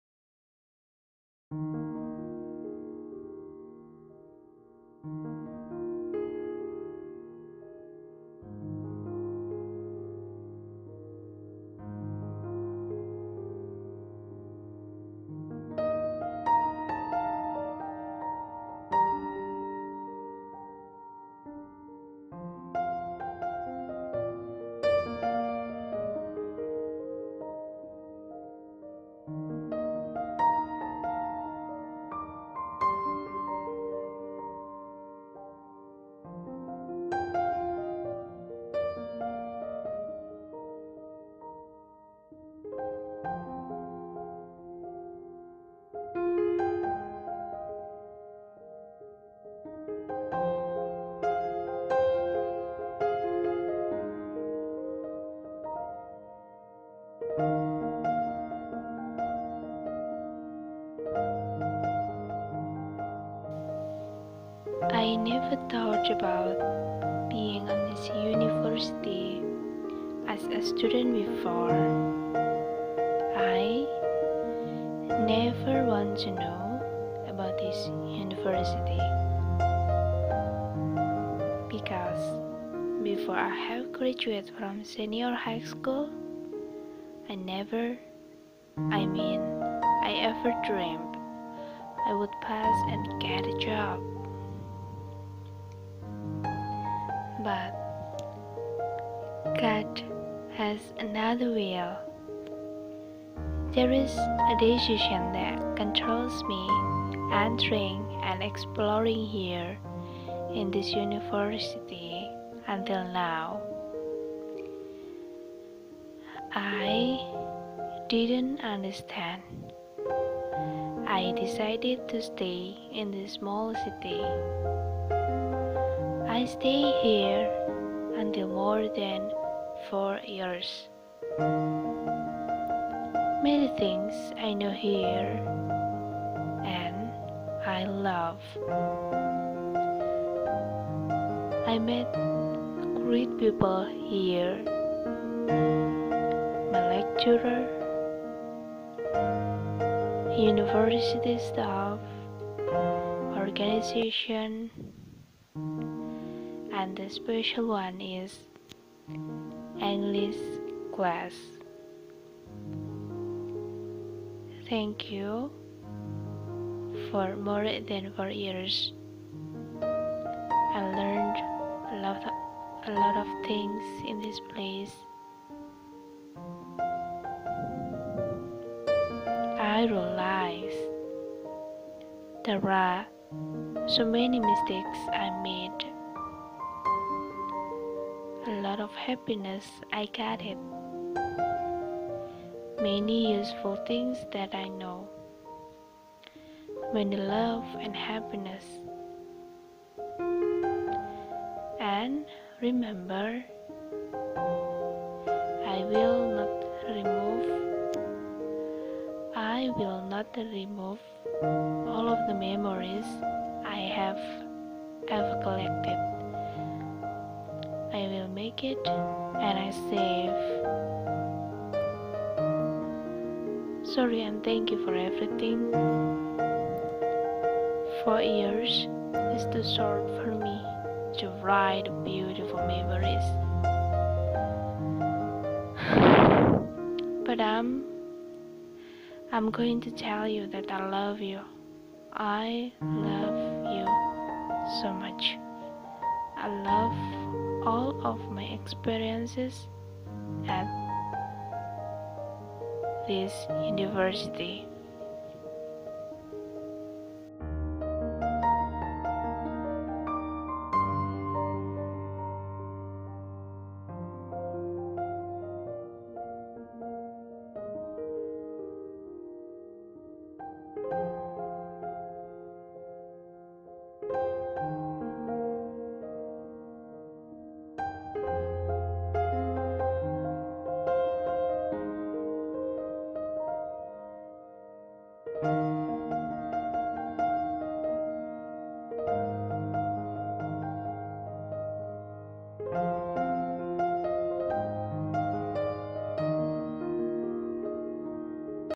Great days when I was at My University. Muhammadiah Univeraity of Ponorogo
Kenangan di unmuh ponorogo

Sorry for my bad voice,
My pronunciation, intonation and grammar.
It's spontan words that I can't expect it before.
Just for our memmories.